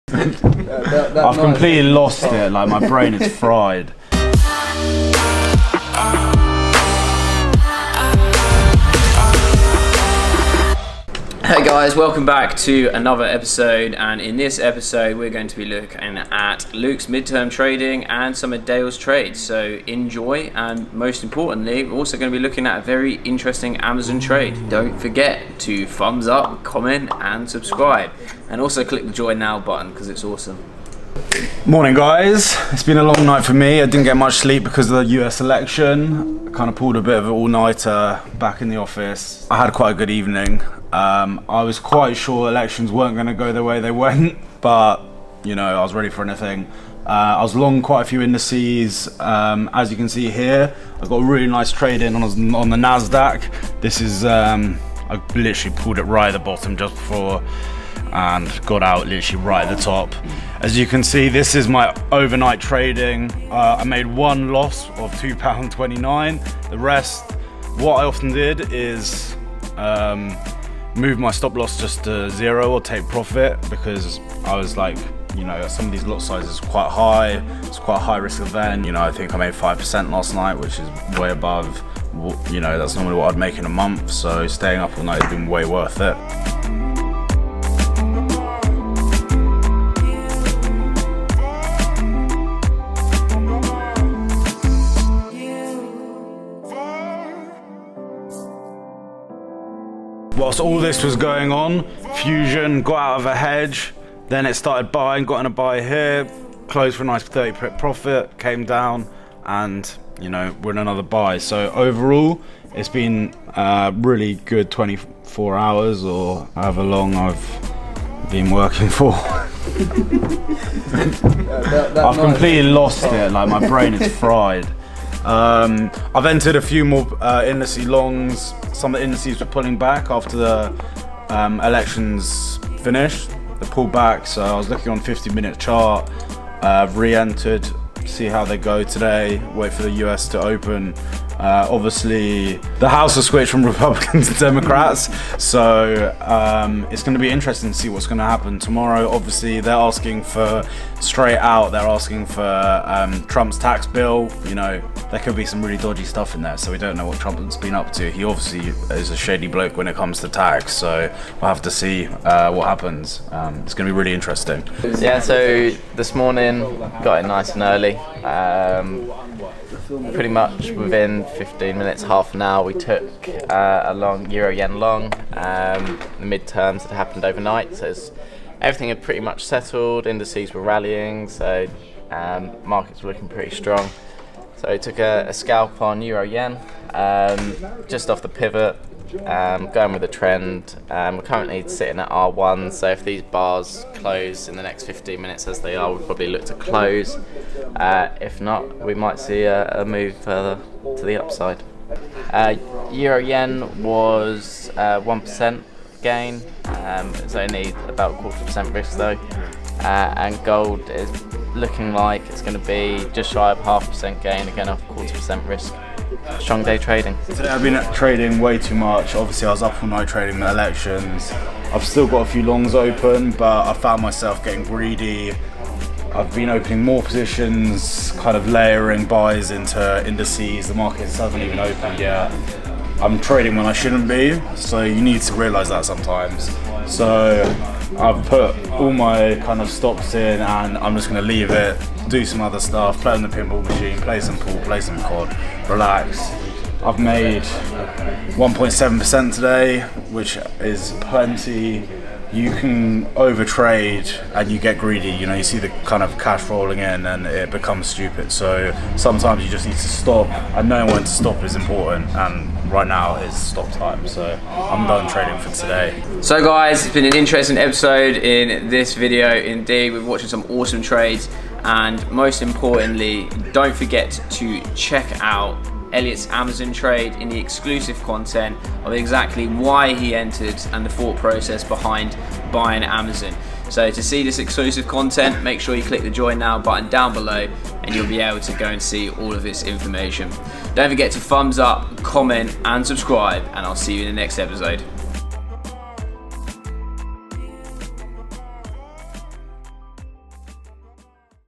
£2300 profit day trading the US midterms
it's been another profitable day. Watch some of the Samuel and Co Traders go through the positions they took within the markets during the US midterm elections.